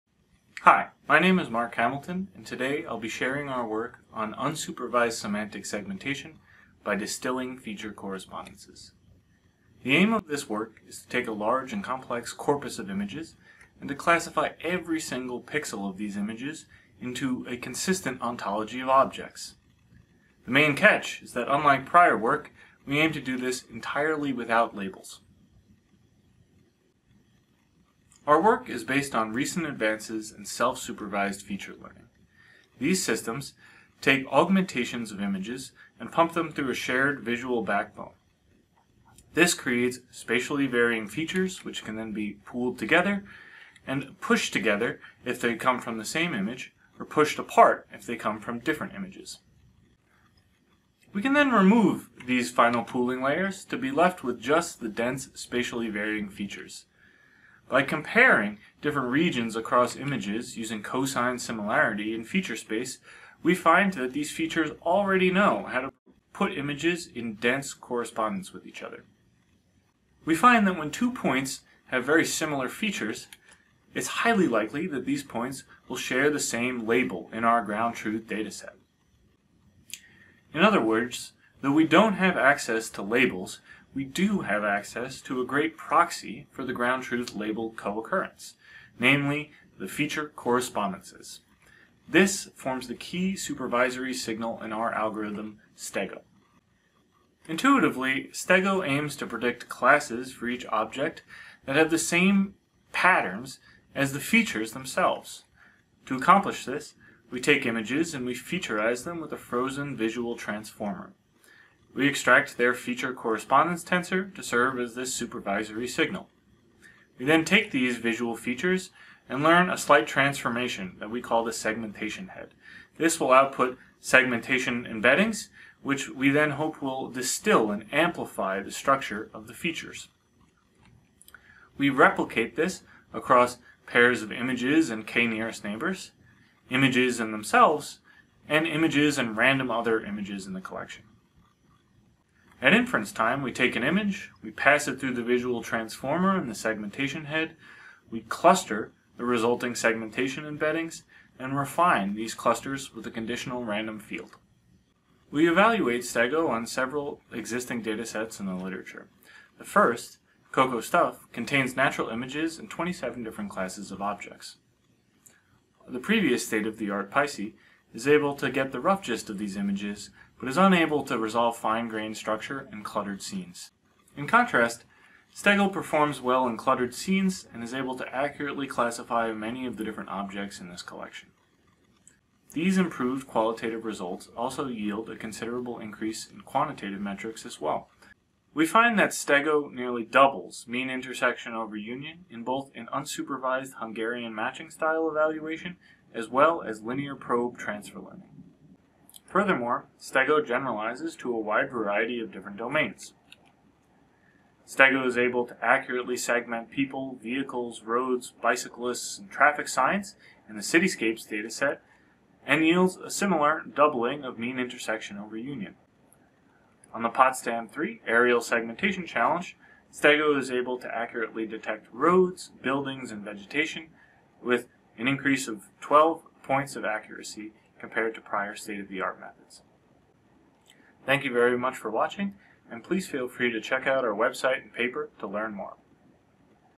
STEGO: Unsupervised Semantic Segmentation by Distilling Feature Correspondences
ICLR 2022 Presentation of the STEGO unsupervised semantic segmentation system.